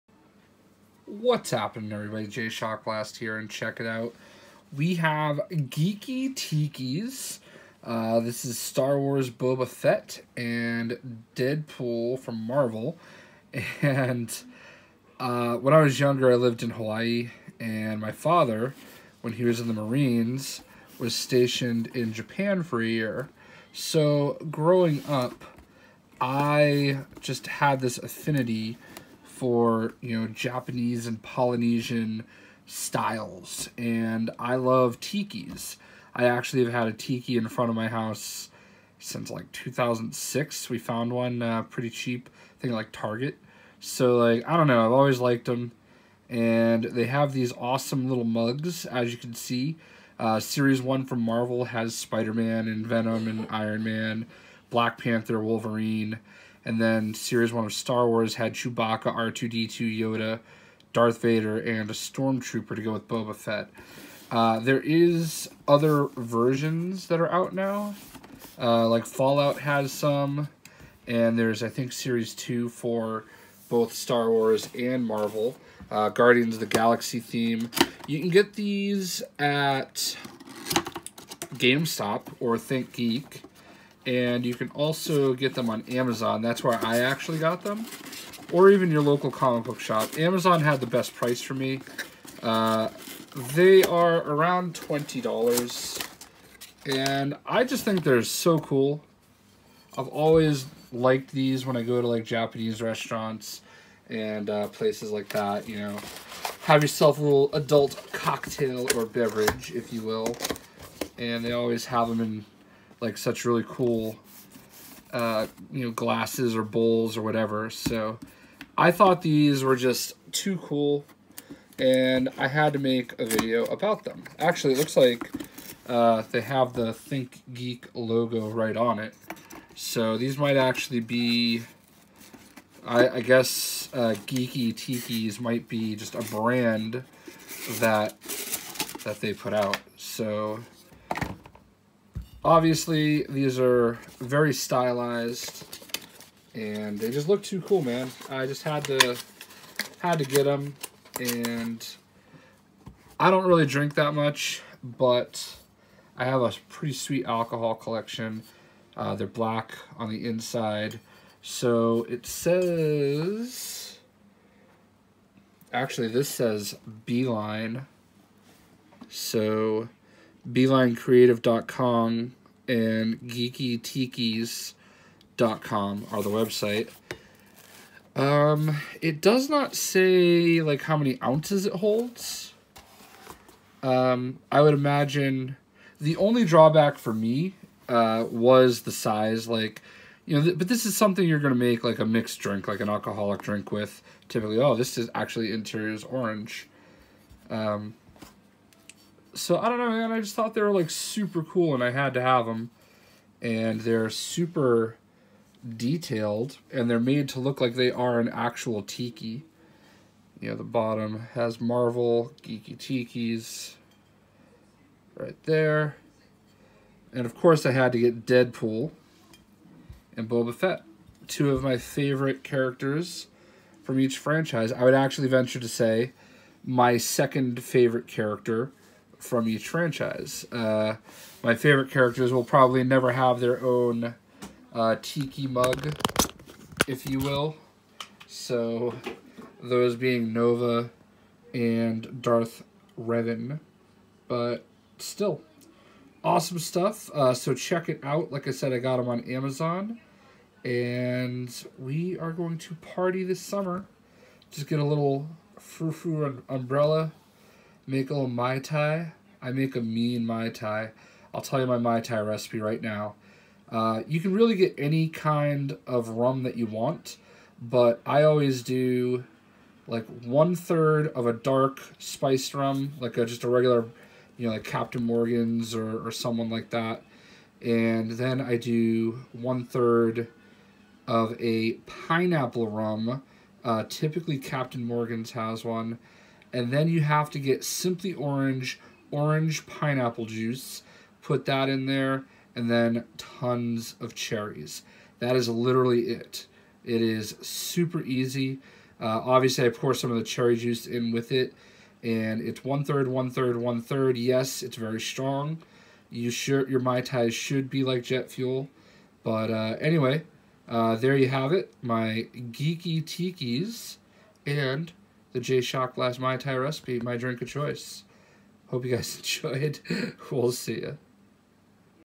Geeki Tikis Marvel’s Deadpool and Star Wars Boba Fett Glass Unboxing
Geeki Tikis Marvel’s Deadpool and Star Wars Boba Fett Glass Unboxing. I HAD to get these. I have a crazy love for Japanese/Polynesian artsy type stuff and tikis in general. I lived in Hawaii and my father was stationed in Japan twice in the Marines, so that probably has a lot to do with it. Anyway, you can find these at Gamestop, Think Geek, Amazon or even local comic book shops. They are really great.